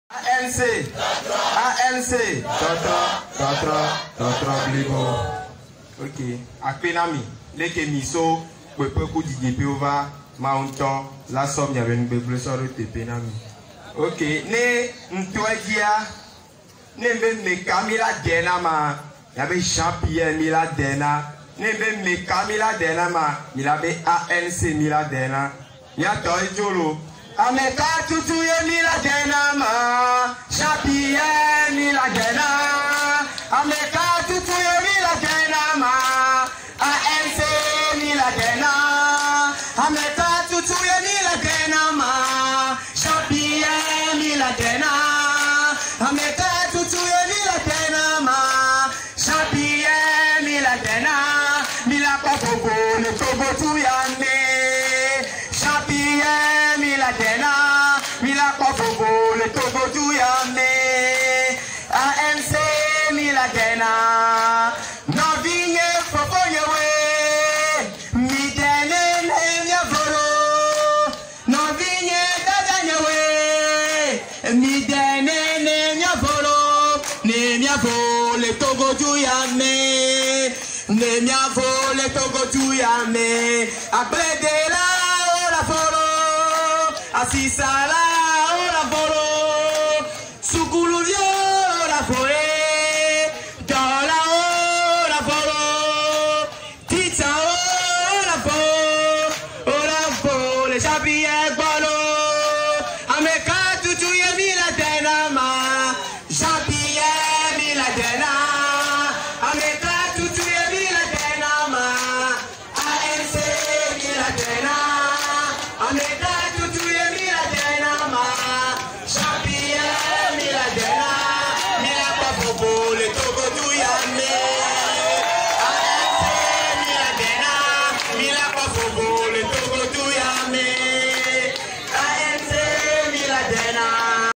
Population d'Aklakou: Méka mila dèna Jean-Pierre Fabre mila dèna ! Méka mila dèna ANC mila dèna !
Campagne présidentielle: Le candidat Jean-Pierre Fabre à Aklakou ce mardi 18 février 2020